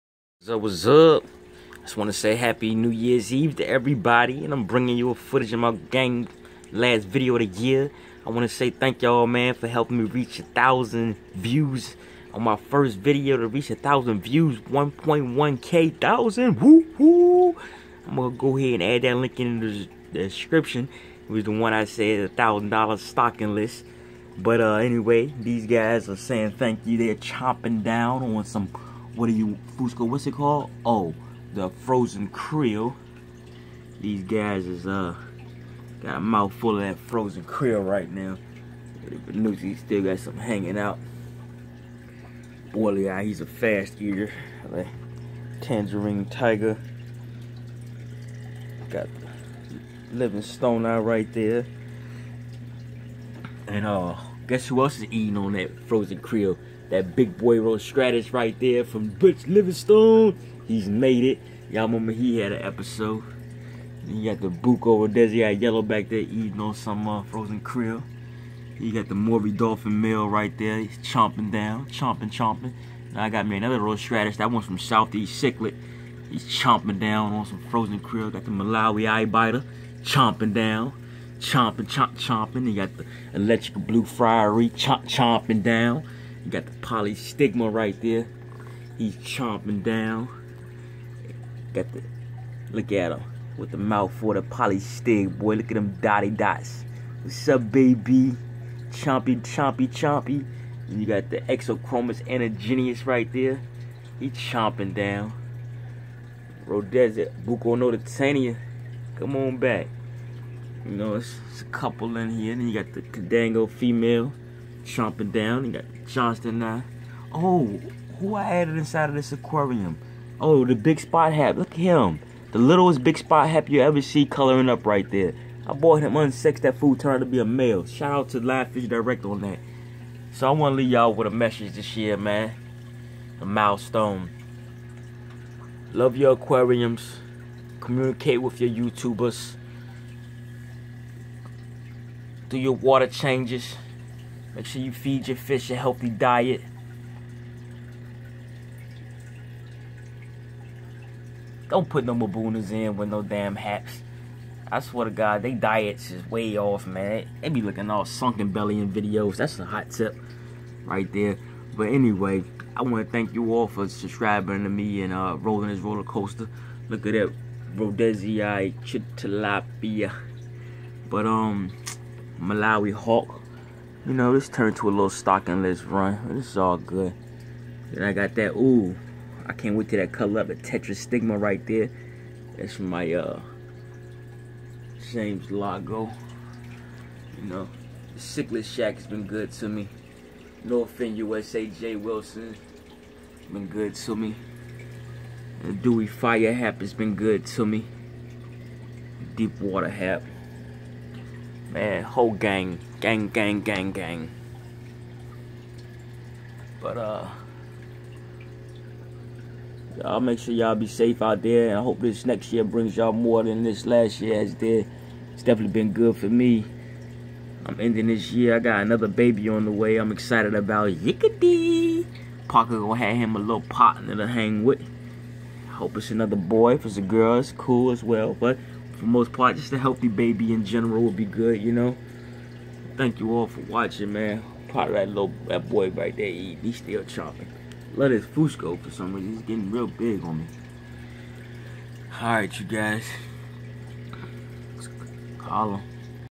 Happy New Years Eve and a BIG THANK YOU! 125 GAL HAP AQUARIUM AFRICAN CICHLIDS AND TIPS
FILTRATION
2 304B 525 GPH A PIECE
3 SUN SUN WAVE MAKERS 900 GPH
CRUSHED CORAL SUBSTRATE 60LBS
HD BACKGROUNDS
125 GAL  6 FOOT AQUARIUM
STOCKING LIST
NIMBOCHROMIS VENUSTUS 2
NIMBOCHROMIS FUSCOTAENIATUS
NIMBOCHROMIS POLYSTIGMA
PLACIDOCHROMIS MILOMO SUPER VC 10
PLACIDOCHROMIS JOHNSTONI
PLACIDOCHROMIS ELECTRA DEEP WATER HAP
ARISTOCHROMIS CHRISTYI
MALAWI THICK LIPS CHILOTILAPIA EUCHILUS
FOSSOCHROMIS ROSTRATUS
CYRTOCARA MOORI BLUE DOLPHINS 2
CHILOTILAPIA RHOADESII 2
BUCCOCHROMIS NOTOTAENIA
BUCCOCHROMIS RHOADESI YELLOW
OTOPHARYNX LITHOBATES YELLOW BLAZE
SCIAENOCHROMIS AHLI ELECTRIC BLU AHLI
HAP PROTOMELAS TEANIOLATUS NDWIE FIRE
PROTOMELAS TANGERINE TIGER
CAPADICHROMIS BORLEYI RED FIN
KANDANGO BORLEYI
DIMIDIOCHROMIS MALAWI EYE BITER
EXCOHROMIS ANAGENYS
Livingstoni
Tetra stigma _______________